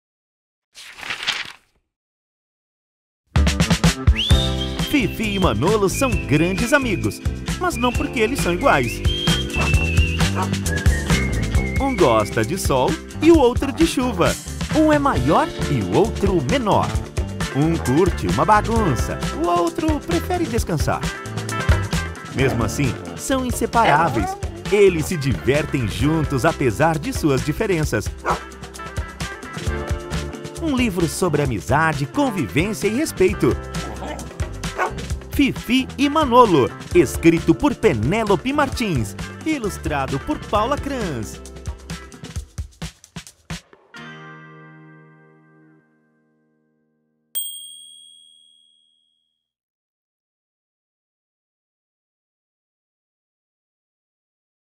Fifi e Manolo
Escrito por Penélope Martins e ilustrado por Paula Kranz, "Fifi e Manolo"  conta a história de uma gata e um cão que dividem a mesma casa, e que mesmo tão diferentes entre si compartilham várias aventuras e não se separam.